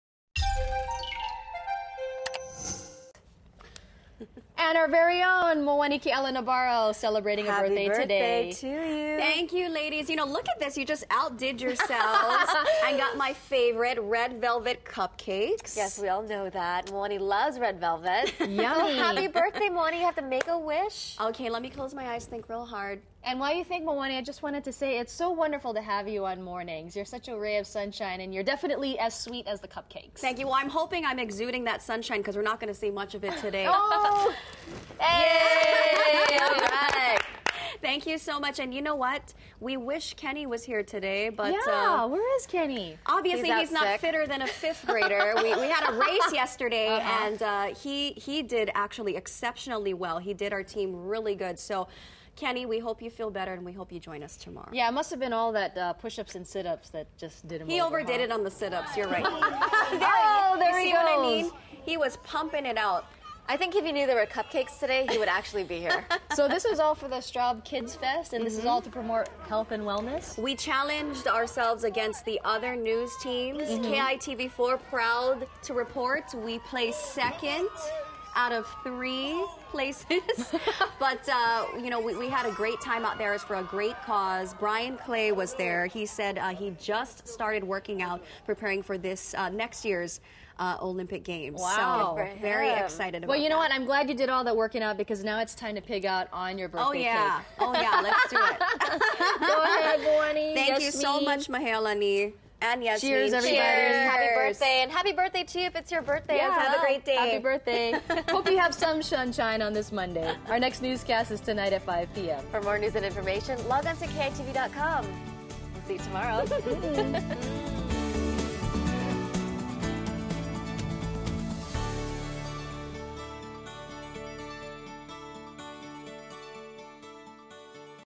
Happy Birthday Moani
It's our morning weather girl's birthday today! For news you need to know,